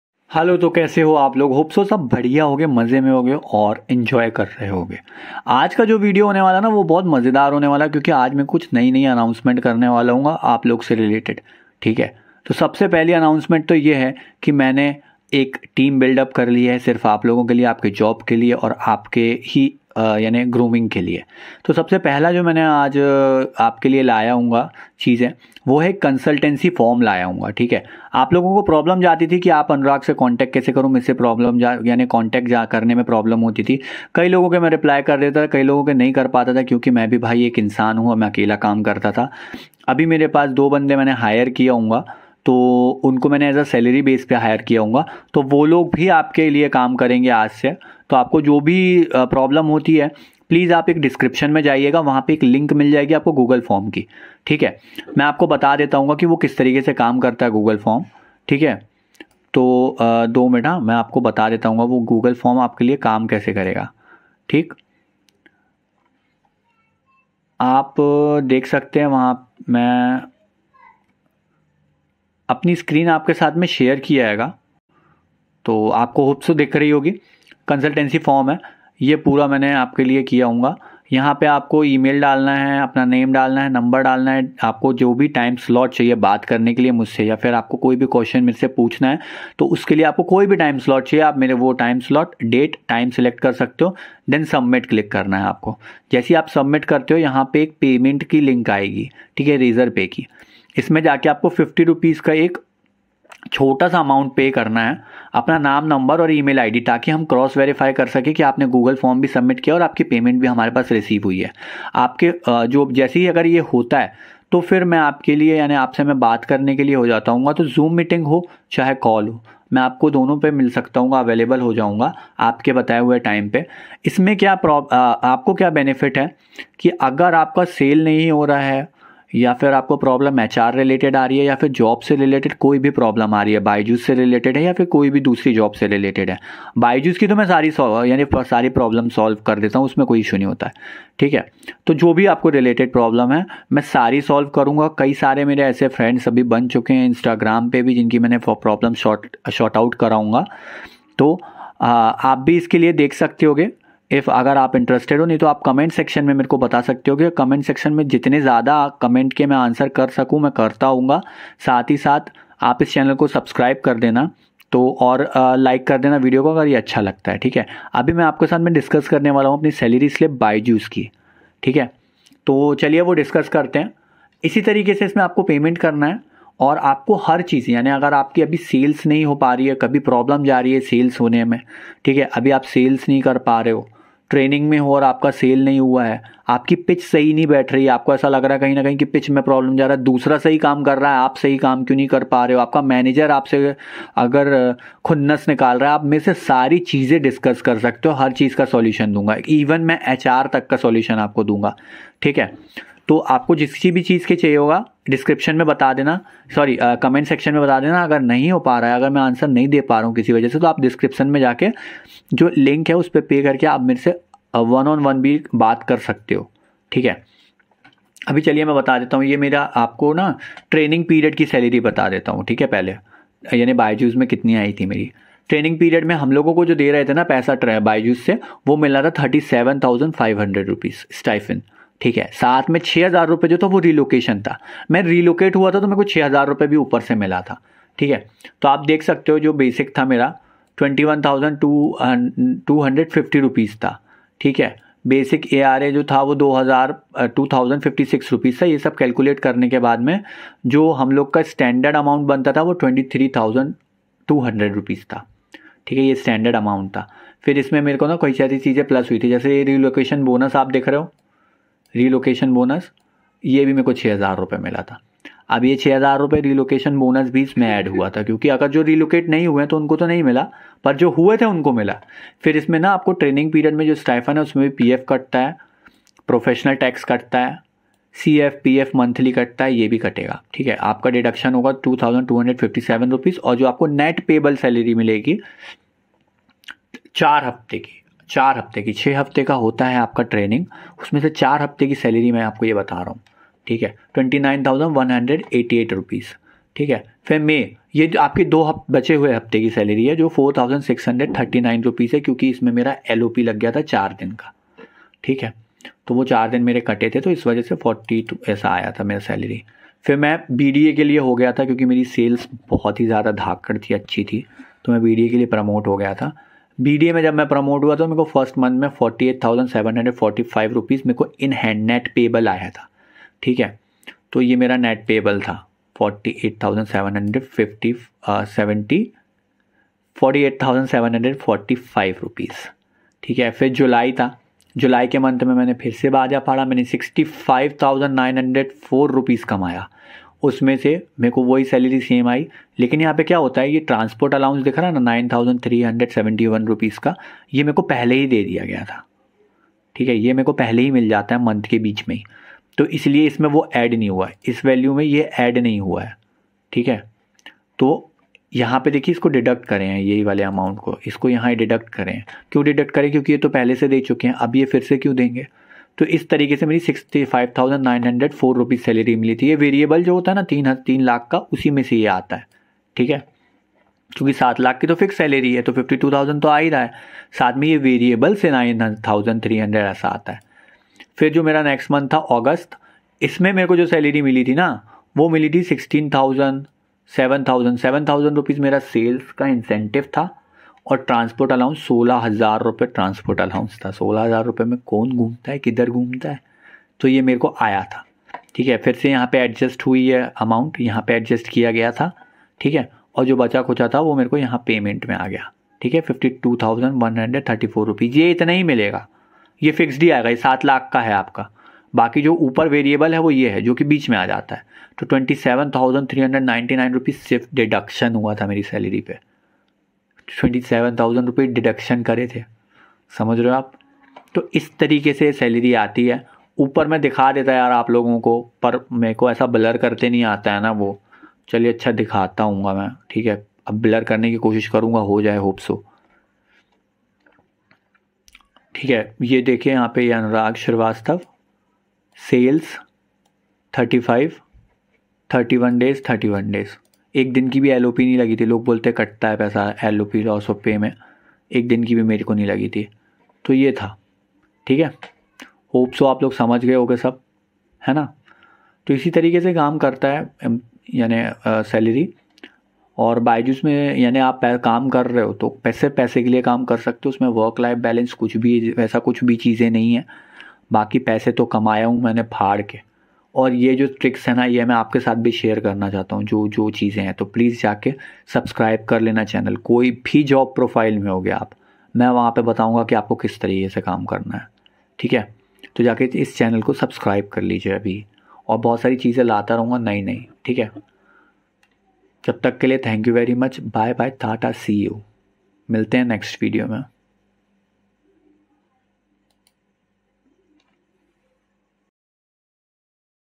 How Many Byjus Pay ??? Salary Slip 80,000 | Byjus CSA In hand Salary | Work from Home Job
How Many Byjus Pay ? Salary Slip 80,000 | Byjus CSA In hand Salary | Work from Home Job | Job For Graduate.

🎯 Job Update
🎯 Job Review in India
🎯 Motivational Video
and many more in-demand.

Thankyou
Regard
Anurag Shrivastava